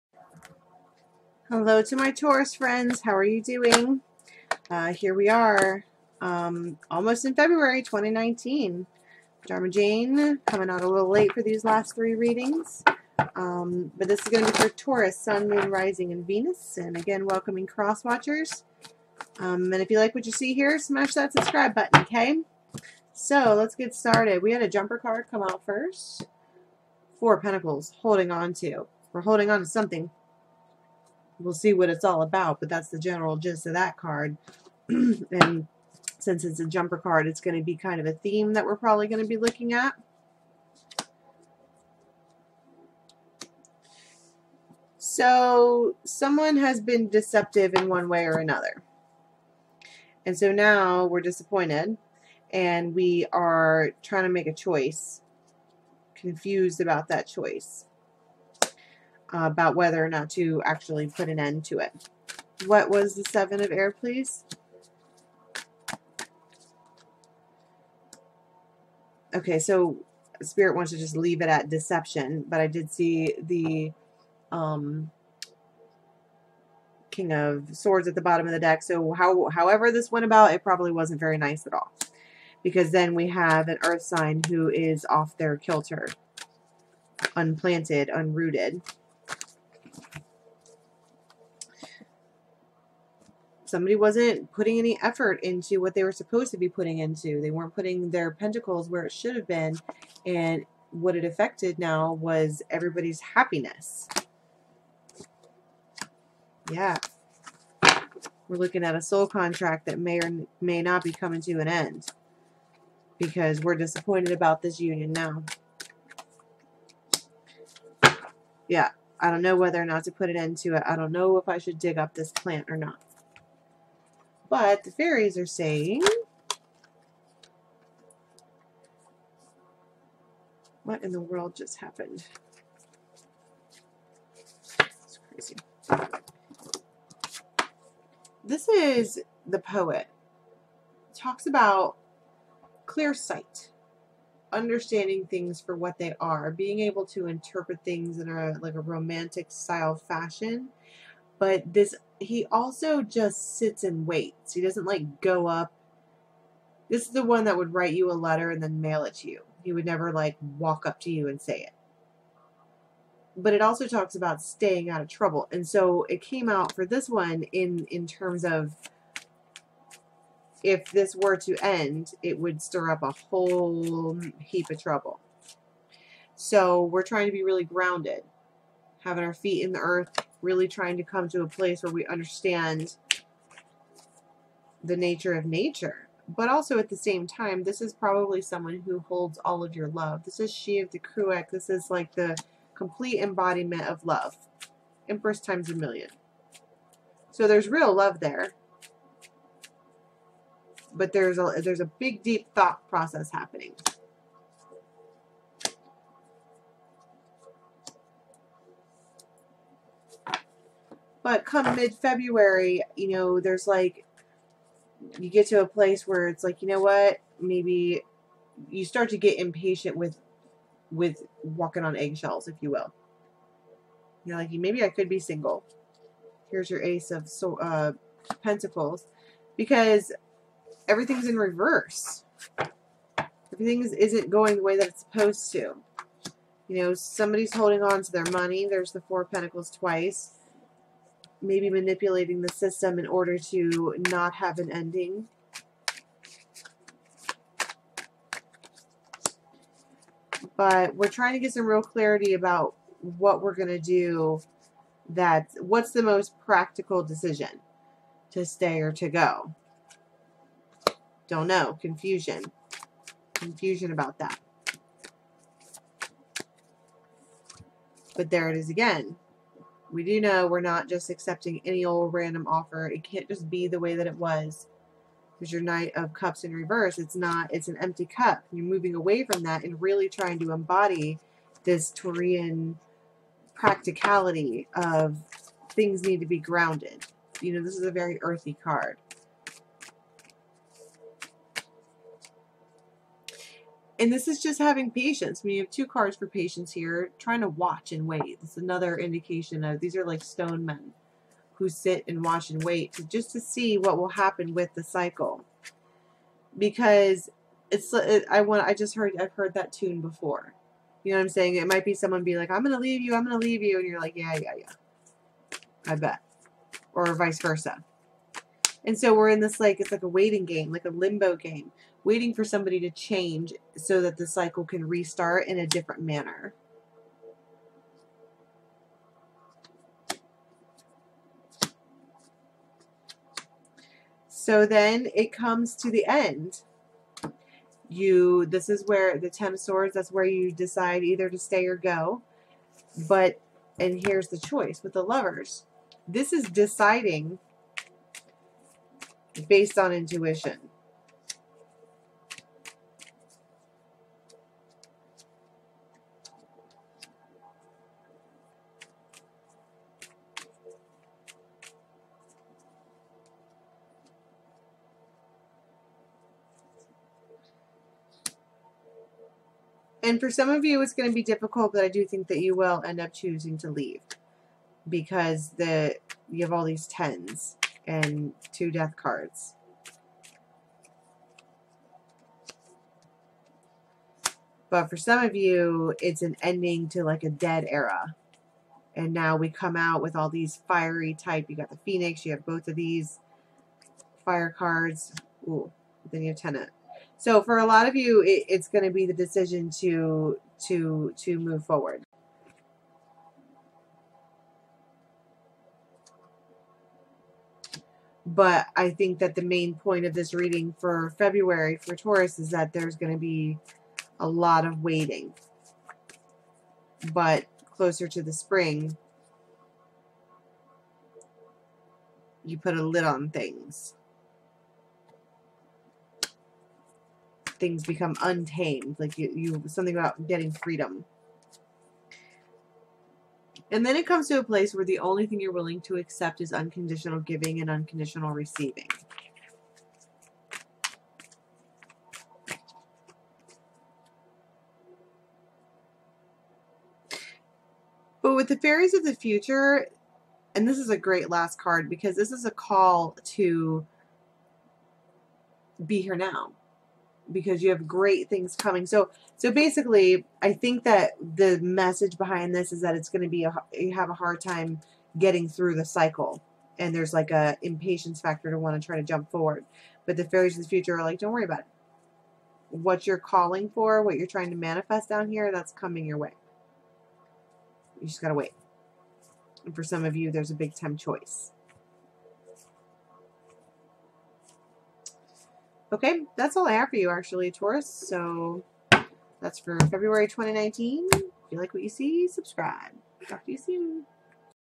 Taurus ~ PATIENCE ~ February 2019 Tarot Card Reading
Greetings Taurus! ~ This month I am bringing you a general tarot reading for your monthly tarot forecast. This spread depicts the recent past, present and possible future outcomes. This reading may not resonate with everyone. The energy present might be for you or someone you know!

CHAT WITH ME LIVE ON KEEN!